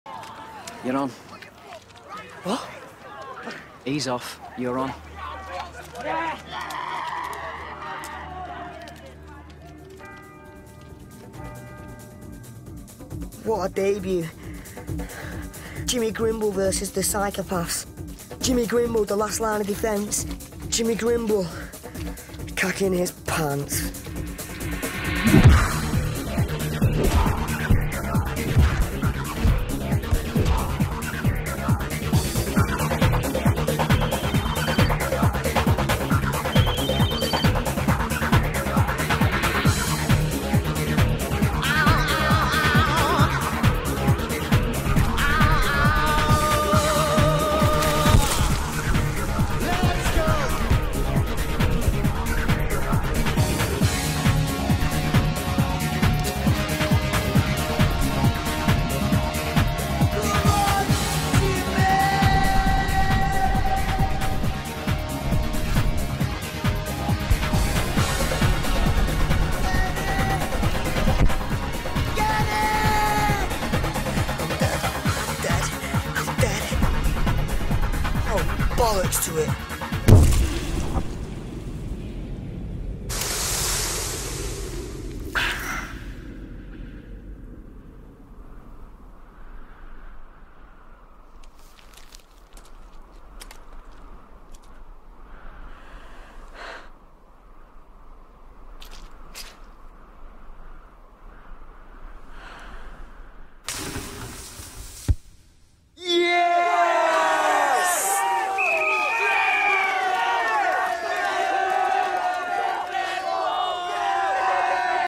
Man City v Stoke 15/09/2012 basketball goal,with frankie goes to hollywood
The legendary Jimmy Grimble scores a cracker.